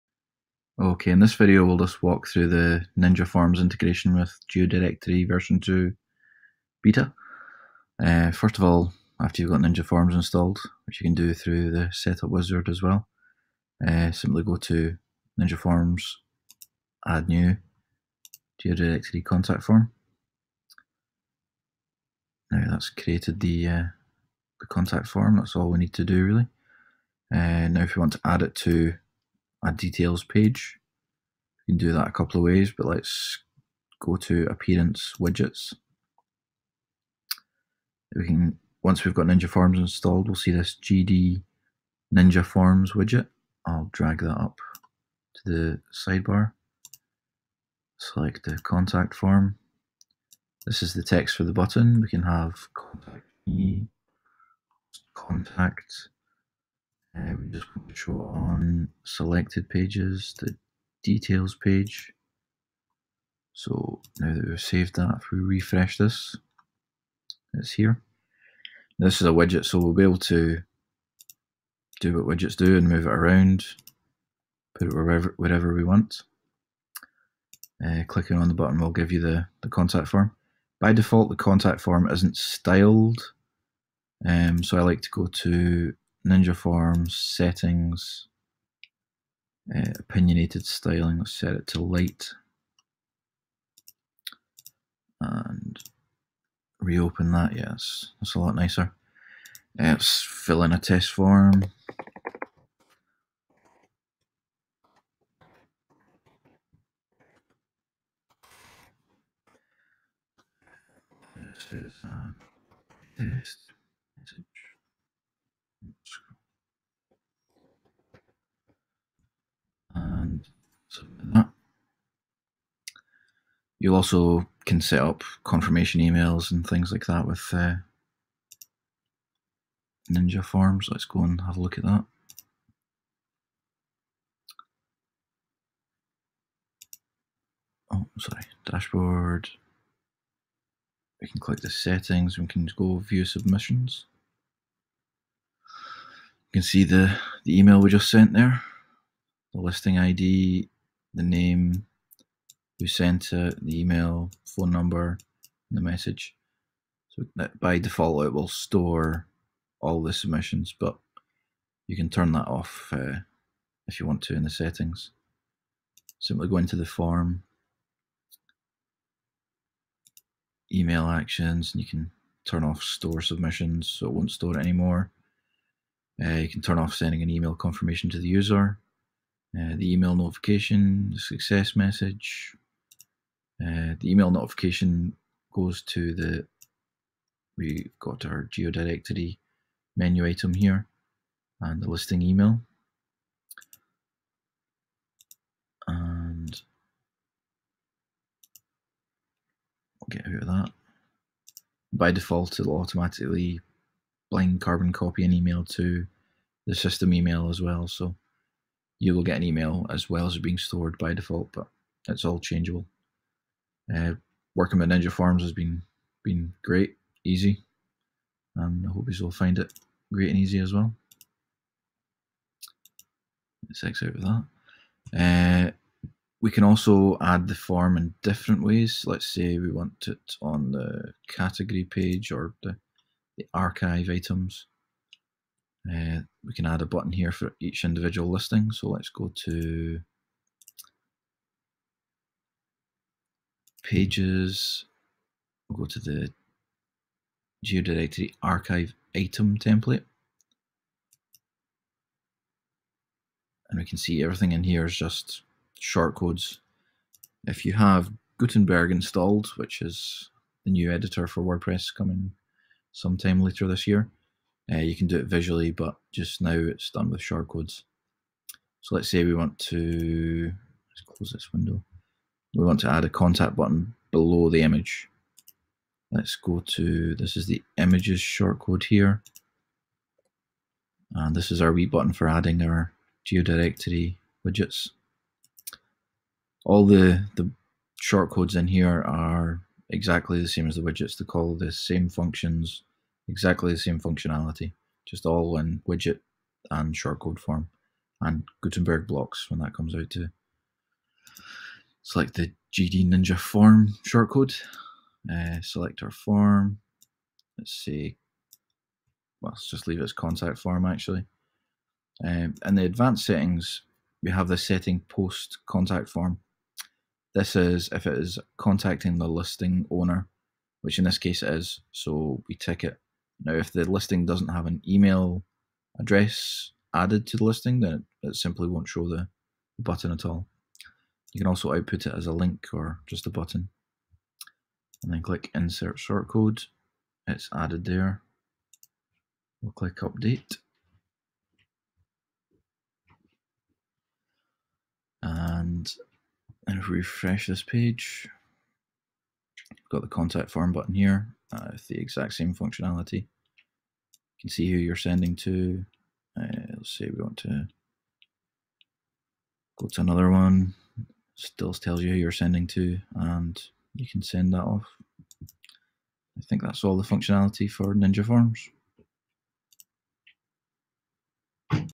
GeoDirectory v2 beta - Ninja Forms Walkthrough
This video runs through the basic setup of Ninja Forms and GeoDirectory v2 beta, you can download it and find the latest info